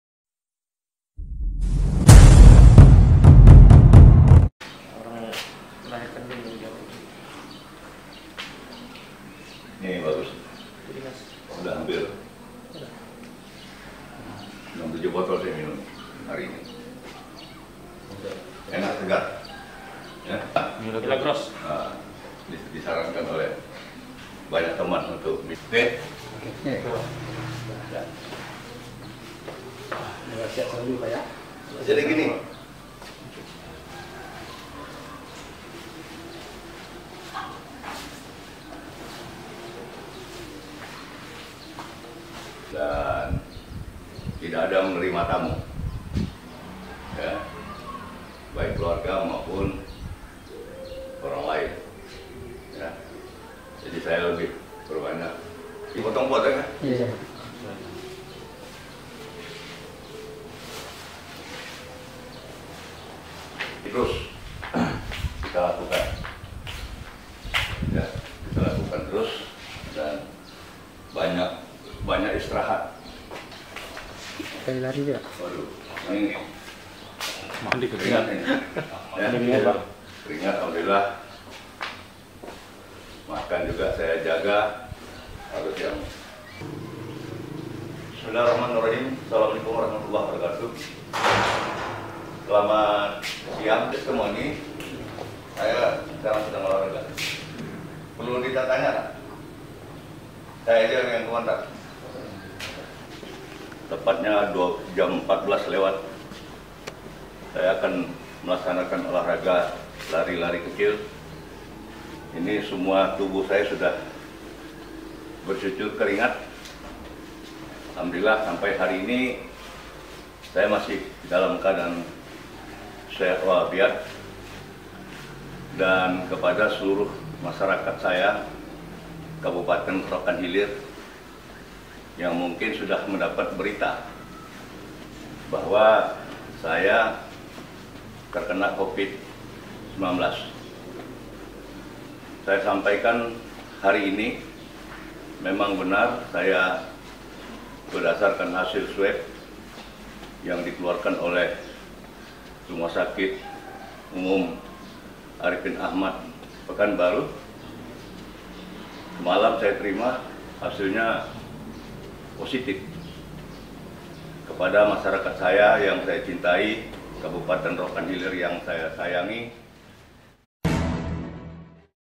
Isolasi Diri Dirumah, Begini Kondisi Bupati Rohil H. Suyatno
ROKAN HILIR - Bupati Rokan Hilir (Rohil) mengharapkan kepada masyarakatnya selalu waspada terhadap corona virus disease 2019 (Covid 19). Dia menjelaskan bahwa memang benar dirinya positif covid 19 berdasarkan hasil swab yang dikeluarkan oleh laboratorium bio molekul rumah sakit Arifin Achmad Pekanbaru. Lanjutnya mengatakan kabar tersebut setelah diperolehnya kemaren, Rabu (02/09/2020) sehingga dia langsung menuju kediamannya untuk mengisolasikan diri.

“Saya tidak ada menerima tamu baik keluarga ataupun orang lain dan olahraga untuk menjaga kebugaran terus kita lakukan. Kemudian banyak istirahat, tambah imunitas tubuh. bahkan makan juga dijaga,”katanya dalam video testimoni yang diterima di Bagansiapiapi, Kamis (3/9/2020).

Dalam video yang berdurasi 03.31 menit di media perpesanan whatsApps tersebut tampak bupati Rohil H.Suyatno minum sebotol cairan dan selanjutnya melaksanakan oleh raga kecil di rumah bertembok warna kuning, kediamannya  yang lama di Jalan Madrasah Bagansiapiapi.

“Semalam saya terima hasilnya positif. Kepada masyarakat yang saya cintai dan kabupaten Rokan Hilir yang saya sayangi, untuk itu saya menghimbau kepada seluruh masyarakat saya yang masih kondisi prima agar menggunakan masker, cuci tangan dan juga jaga jarak,”katanya.

Dia menjelaskan dalam testimoninya bahwa dirinya hingga saat ini dalam kondisi sehat. Dikatakannya bahwa tadi pagi (Rabu, 03/09/2020,red) dia menjemur badan sambil olehraga kecil kemudian istirahat dan selanjutnya sore hari ini jam 14.00 wib dia juga melaksanakan olah raga.

“Alhamdulillah hari ini saya kondisinya sangat prima dan kita menjaga kebugaran kita sehingga kita fit terus,”ujarnya.

Dalam kesempatan ini bupati Rohil H.Suyatno menyampaikan kepada khalayak ramai yakni masyarakat Rokan Hilir agar selalu waspada bahwa covid 19 melanda indonesia, provinsi Riau khususnya kabupaten Rokan Hilir.

“Sekali lagi saya dalam kondisi sehat dan menganjurkan kepada masyarakat untuk tetap waspada, tetap lakukan menjaga jarak dan pakai masker yang jangan sekali-kali ditinggalkan. Karena dengan cara itulah kita bisa menyelesaikan atau menghilangkan virus corona yang mungkin dengan sengaja atau tidak sengaja itu akan menjalar kemana-mana. Sekali lagi saya berpesan kepada masyarakat saya Rokan Hilir untuk selalu waspada terhadap covid 19 di kabupaten Rokan Hilir yang kita cintai ini,”katanya memungkasinya. (Rls)